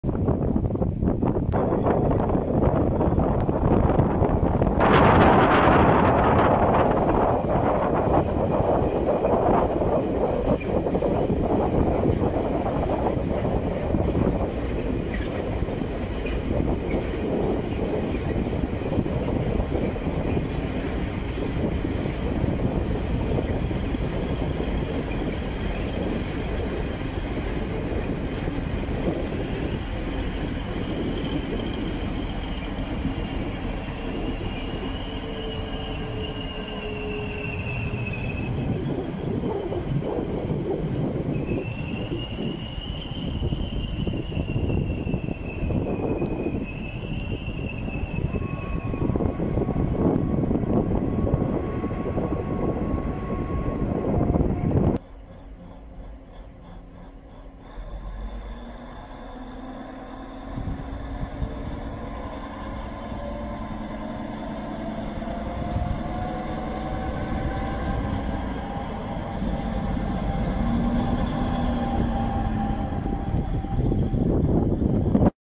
TRENO MERCI CON TRAMOGGE IN MANOVRA A ROBILANTE (CN) LUN. 4 - 11 - 2019
MANOVRA AL CEMENTIFICIO DI ROBILANTE CON IL TRENO MERCI PROVENIENTE DA TRINO VERCELLESE CON LE SUE TRAMOGGE CARICHE DI KLINKER UN MINERALE CHE SERVE PER FARE IL CEMENTO!